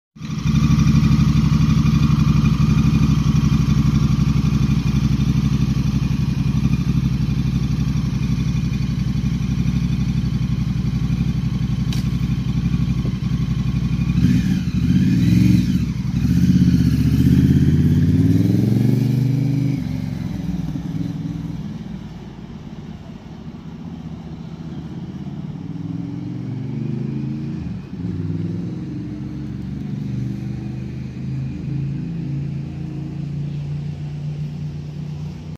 Another member rejoins our stable.
Airman CJ gets her Rebel 1100 delivered! And demonstrates her innate ability for riding. SMOOTH.